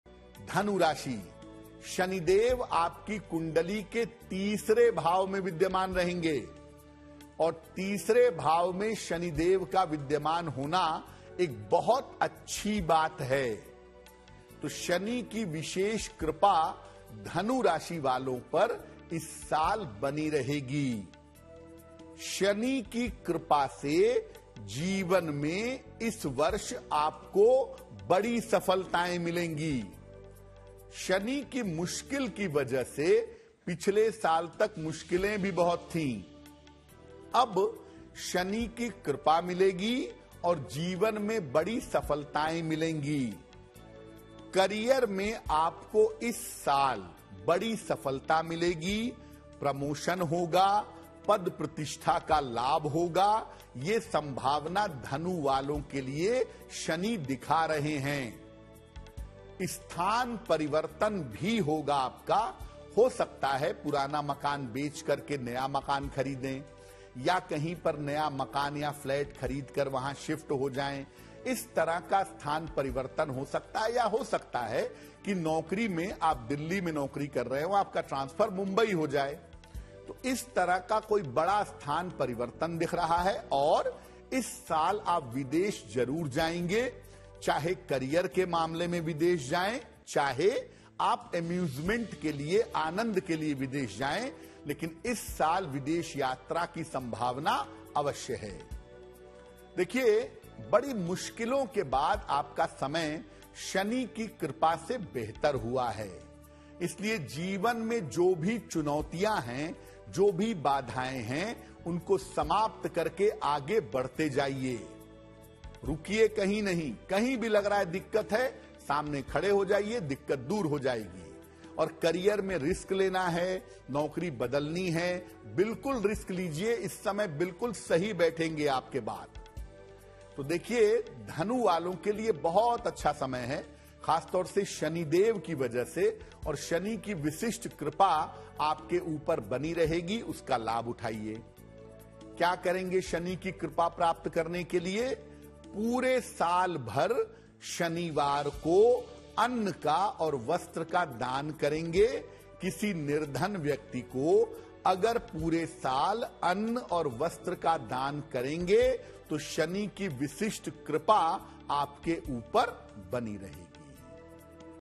Shani Dev 2024: शनि देव का रहेगा साल 2024, धनु राशि के लोगों को मिलेंगी बड़ी सफलतायें
इस वीडियो में पंडित शैलेंद्र पांडेय बता रहे हैं कि साल 2024 में धनु राशि पर शनि का क्या प्रभाव रहेगा. ज्योतिष के मुताबिक, धनु राशि के जातक हर बाधा को समाप्त करके जीवन में आगे बढ़ते जायेंगे. पूरे वर्ष शनिवार को अन्न और वस्त्र का दान करें.